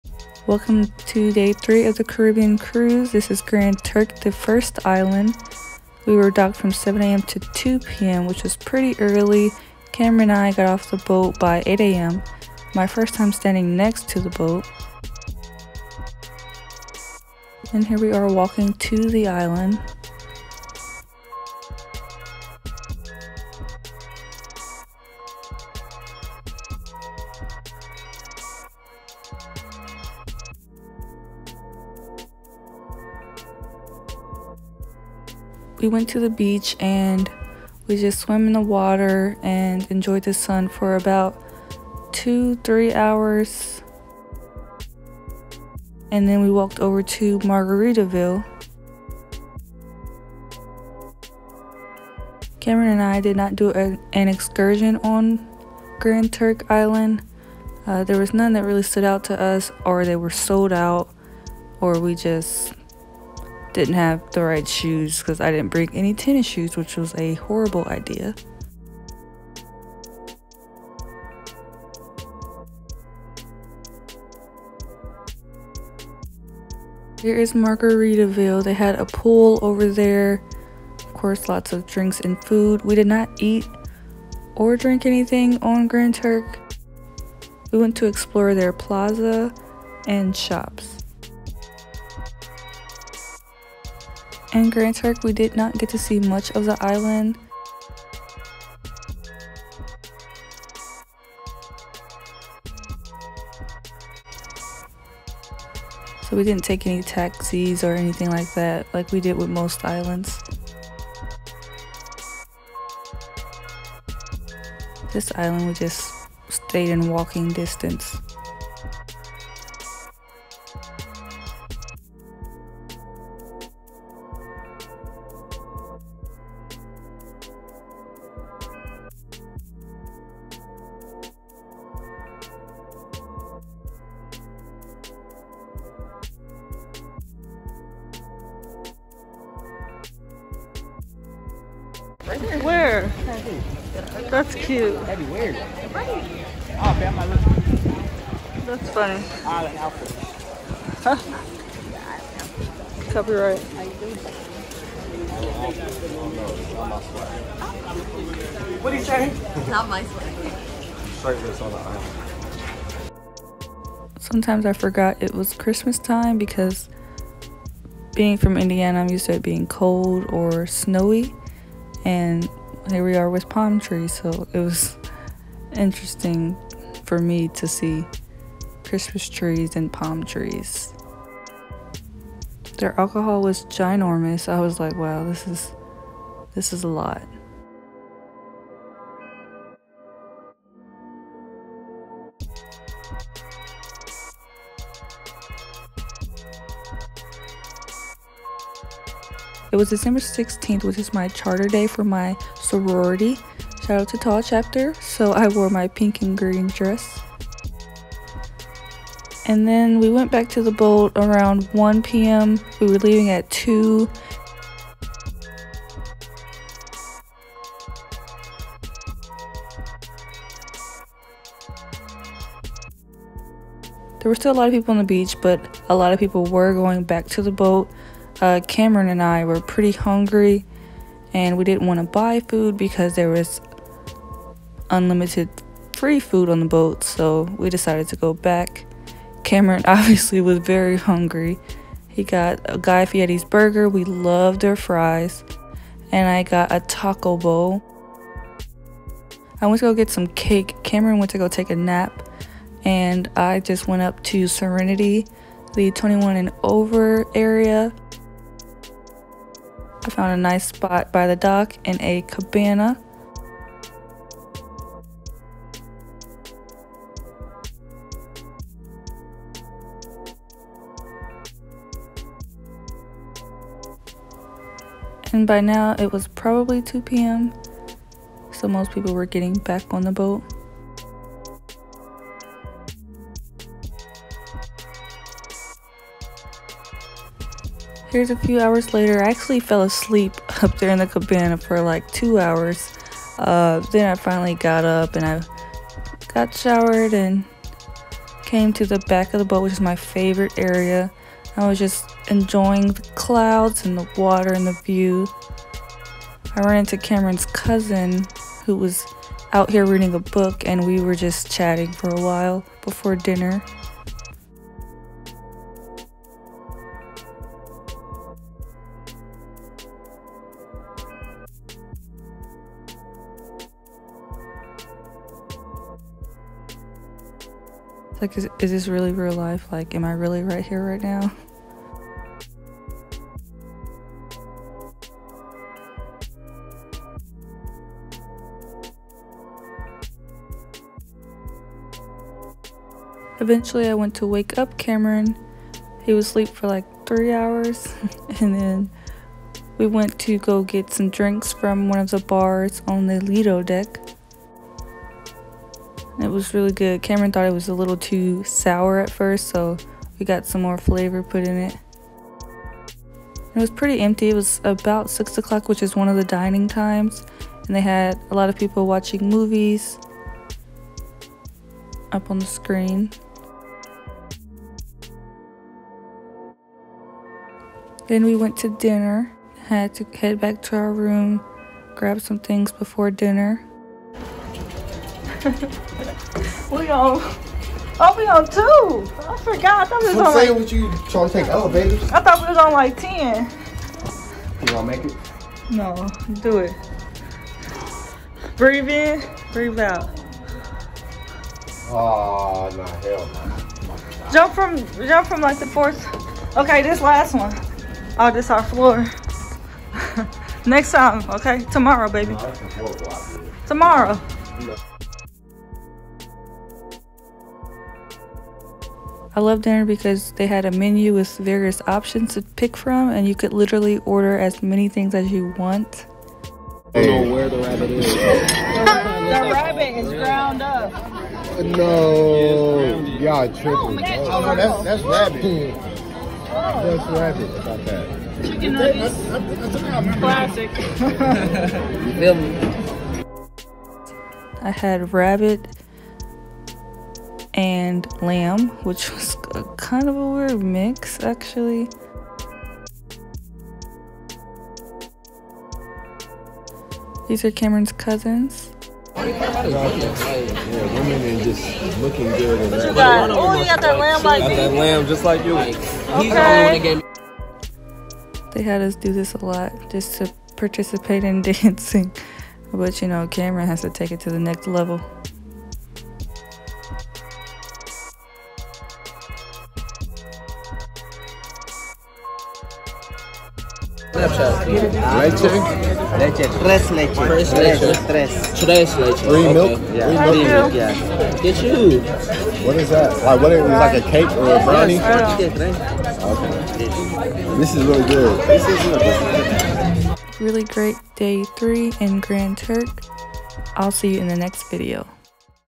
Grand Turk Day 3 | 8 Day Carnival Caribbean Cruise Vlog
Hey Youtube!

This is day 3 of the carnival cruise. Today we are in Grand Turk!! Beautiful ocean that i swam in for hours. We did not take a taxi anywhere, or do any excursions for this island. I later learned this was the only island that docked right on the beach! I appreciated that lol. I had a great tine, but would definitely explore more next time.

xoxo,
Allie